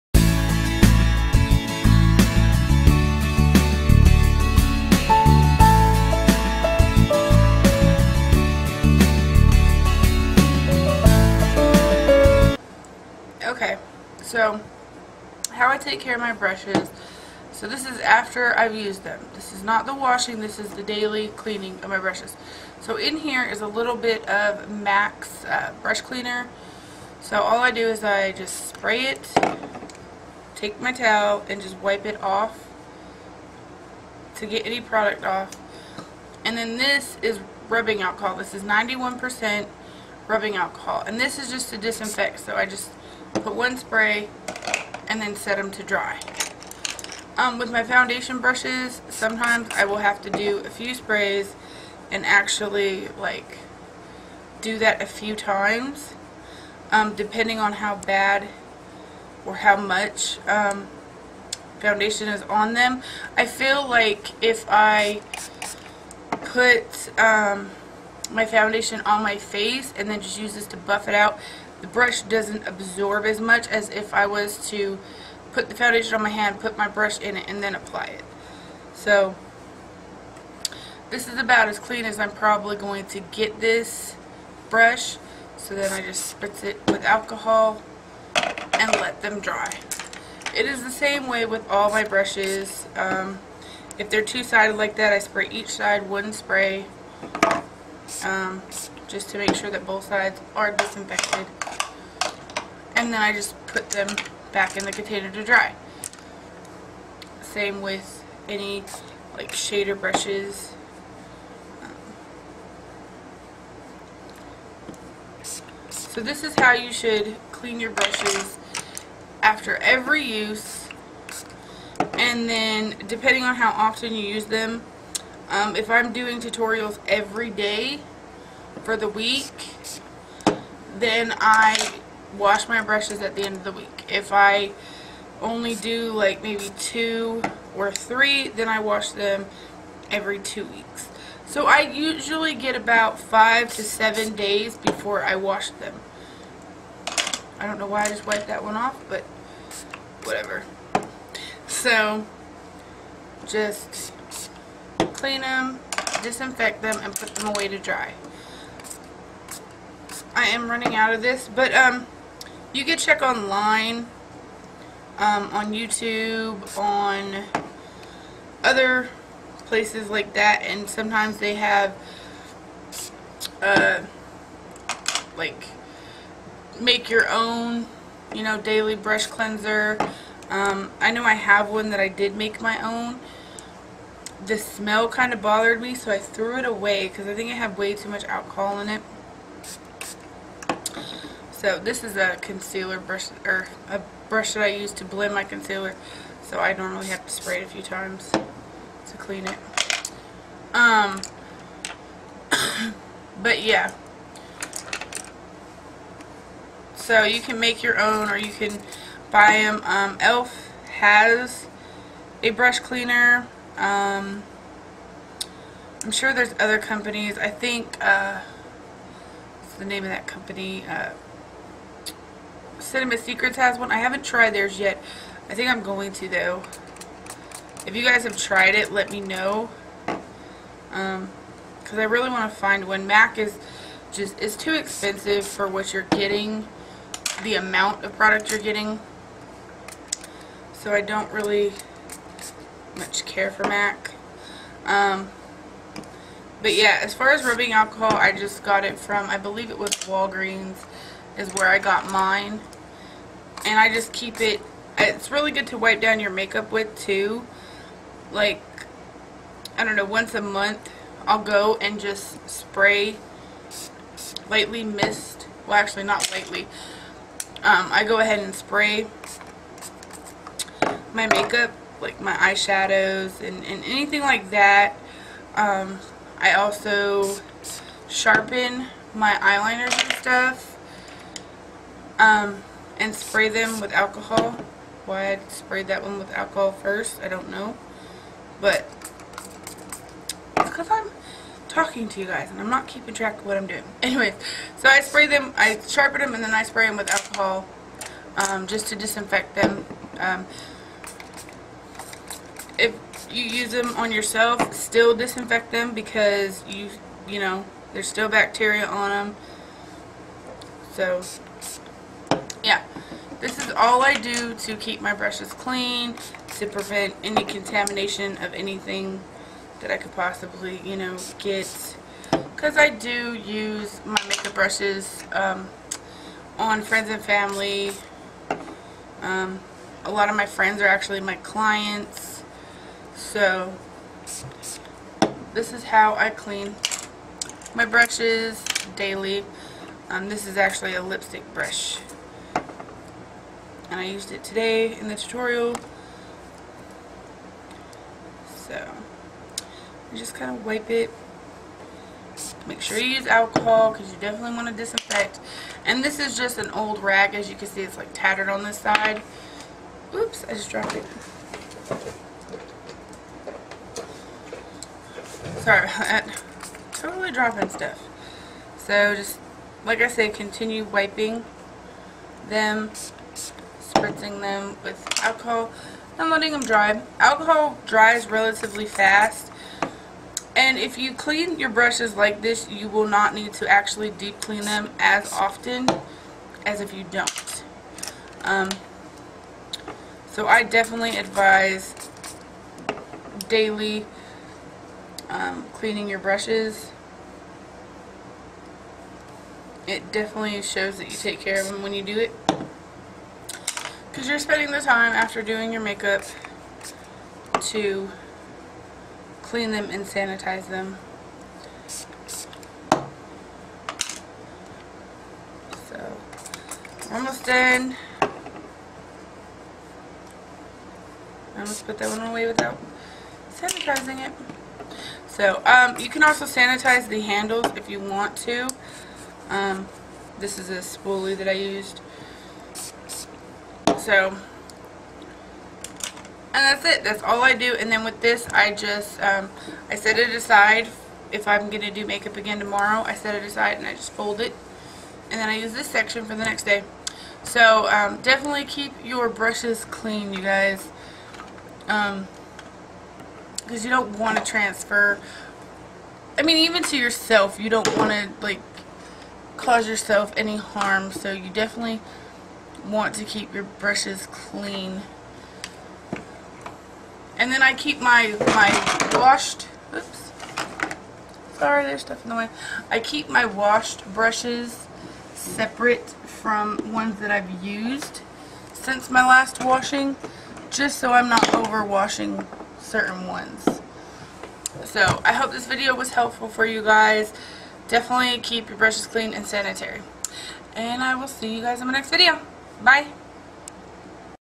How Clean and Disinfect Your Brushes Daily/After Ever Use
Hey my B-E-A-U-tiful people,
Today's video is about keeping your brushes cleaned and disinfected. I am an avid cleaning and disinfecting person. This is very important even if you only use the brushes on yourself. If you don't clean your brushes all types of bacteria can form which can cause anything from acne to fungus. Please keep your brushes clean. You do not need MAC brush cleanser there are plenty of videos on youtube of how to clean your bushes. I will be posting a video soon of my DIY brush cleanser.

Thank you for being the most supportive subscribers a person could ask for!

"There is no cosmetic beauty like loving yourself."